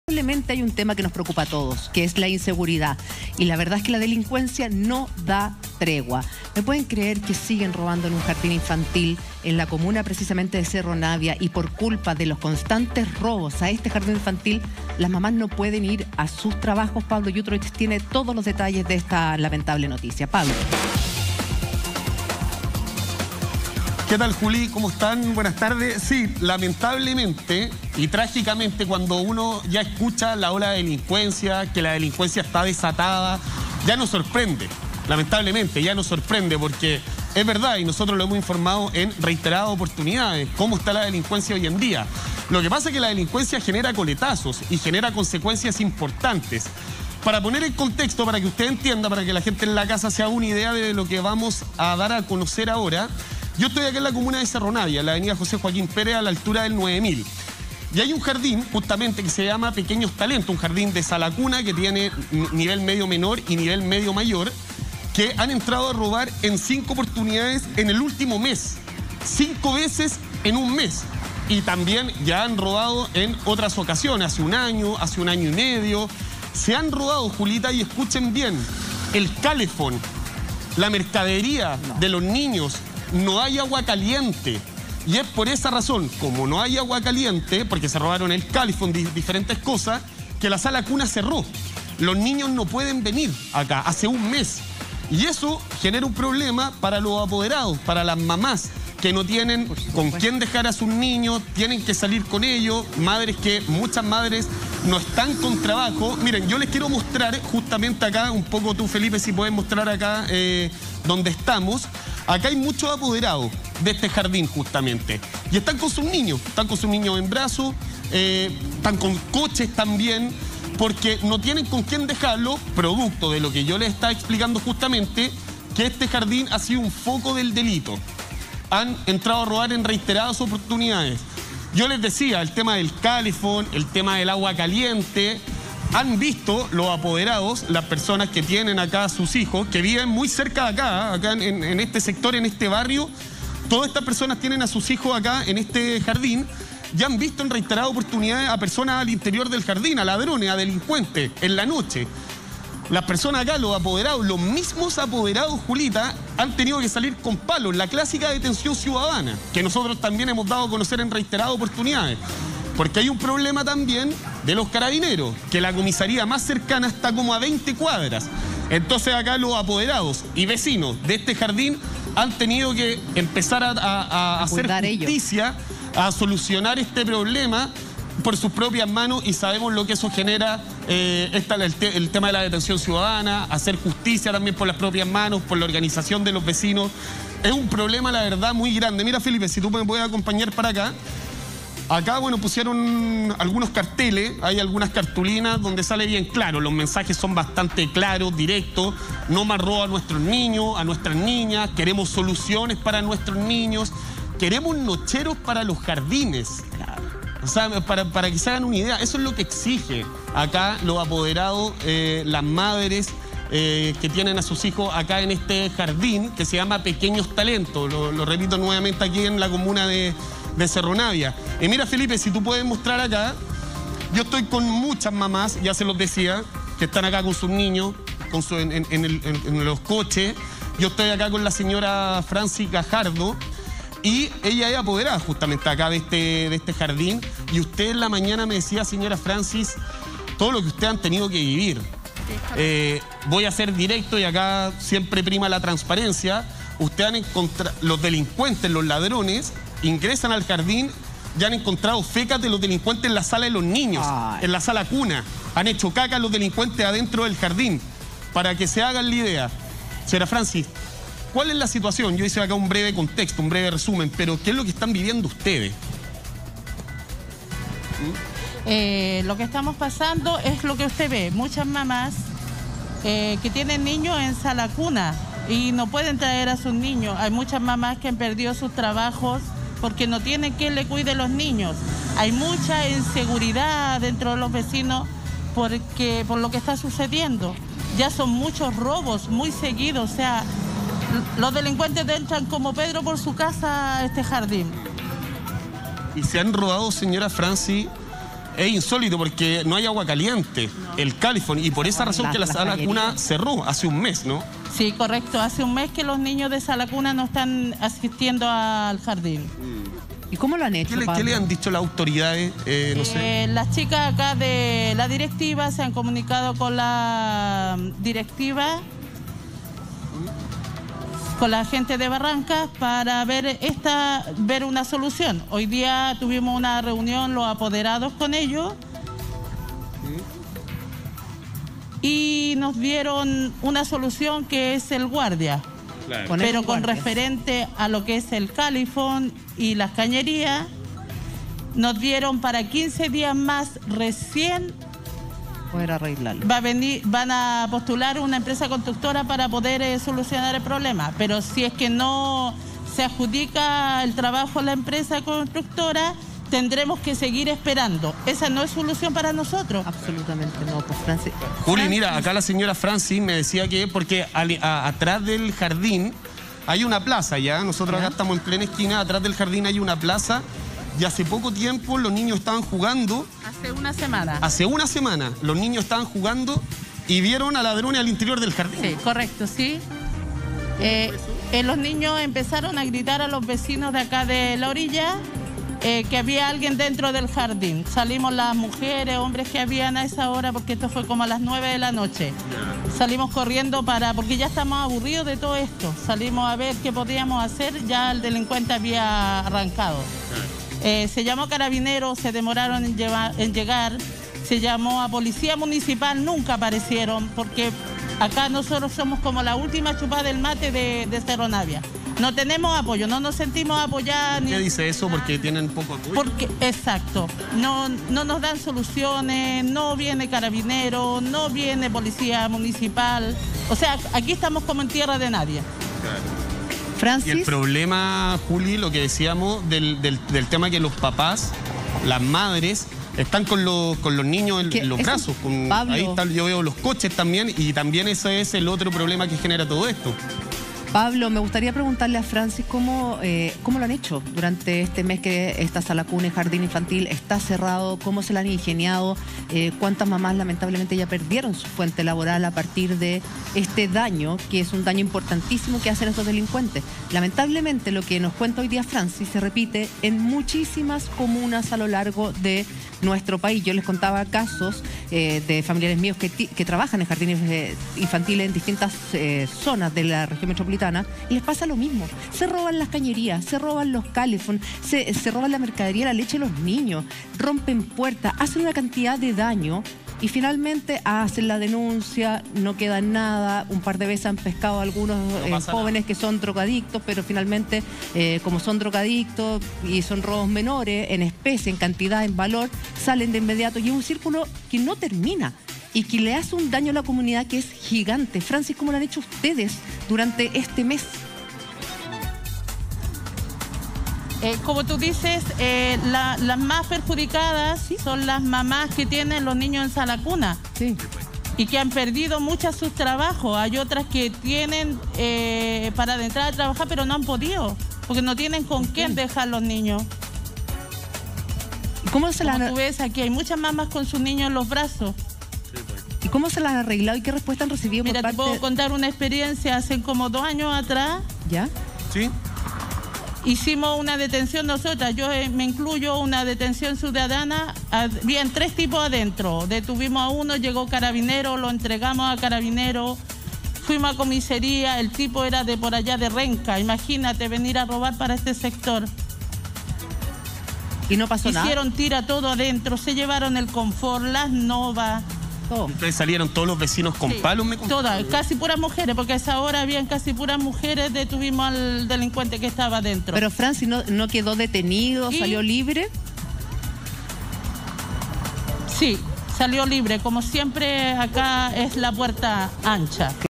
Cinco robos provocaron la clausura de jardín infantil municipal en Cerro Navia
Apoderadas de "Pequeños talentos" manifestaron su molestia y preocupación ante la delincuencia que ha afectado a este establecimiento municipal, ya que delincuentes se han robado hasta las cañerías, lo que provocó el cierre del lugar.